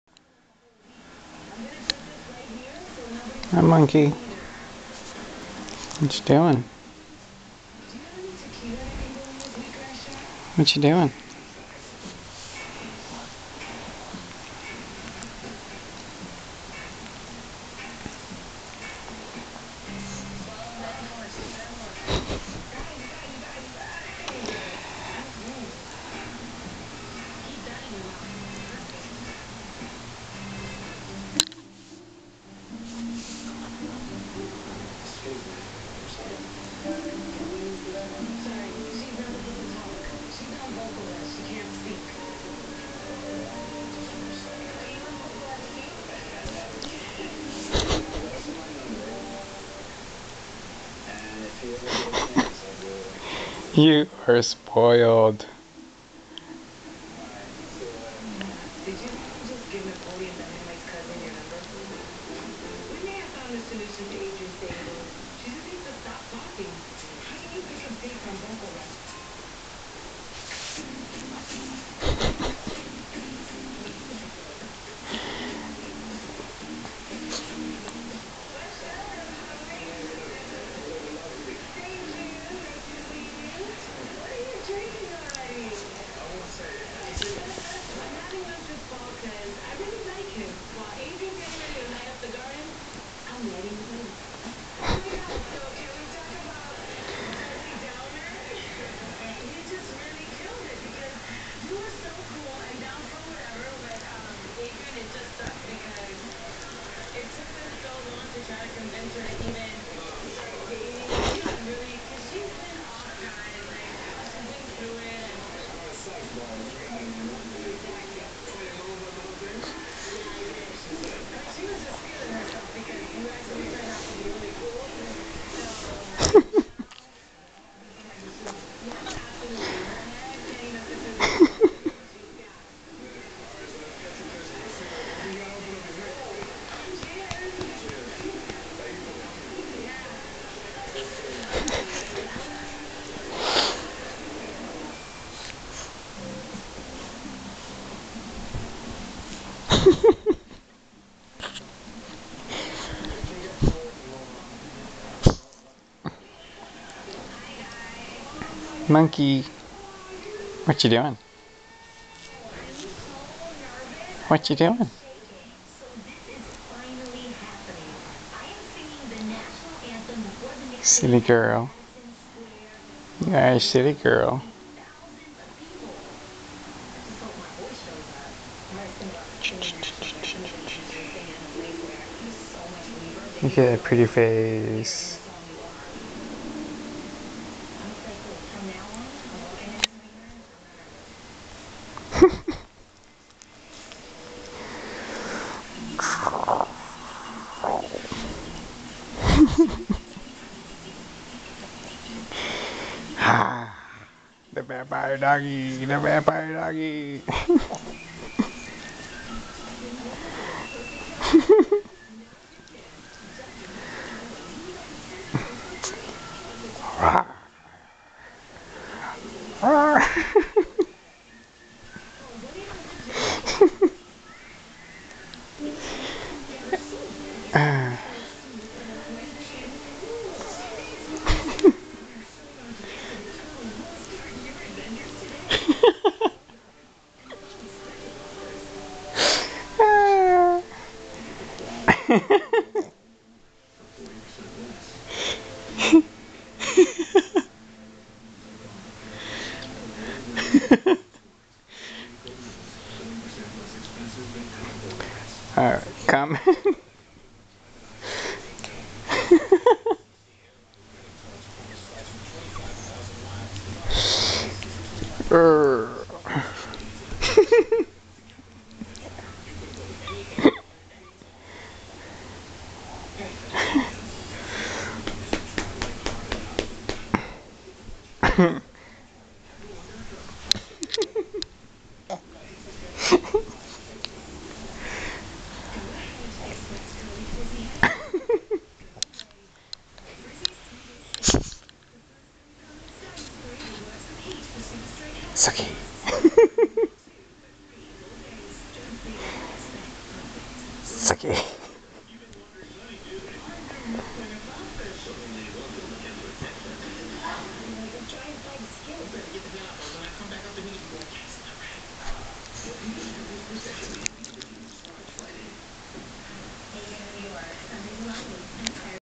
Playin with Sookie
Sookie loves her head rubs... She keeps nudging my hand to pet her every time I move it away :) then she attacks my hand like she is a Vampire... LOL